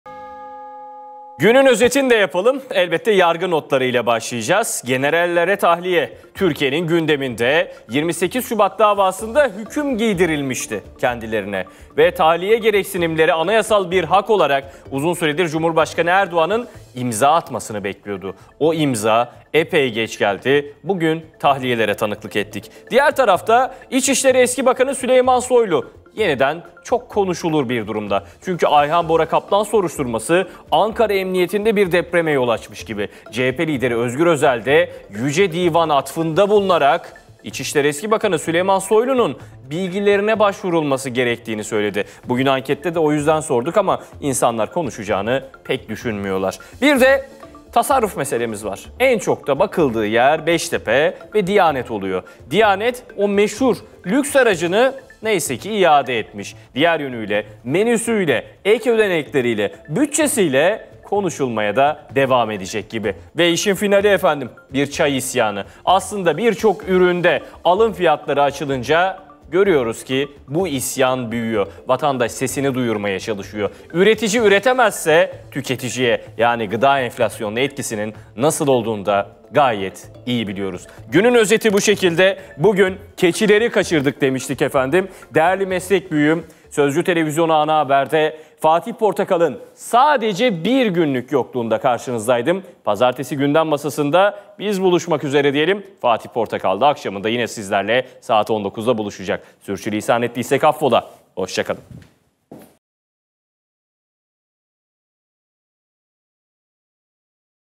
Can Coşkun'dan Gündemin Özeti! 28 Şubat Sanıklarına Tahliye, Süleyman Soylu Gündemden Düşmüyor
Digiturk: 60
D-smart: 93
Turkcell TV Plus: 69
Tivibu: 69
Türksat Kablo TV: 76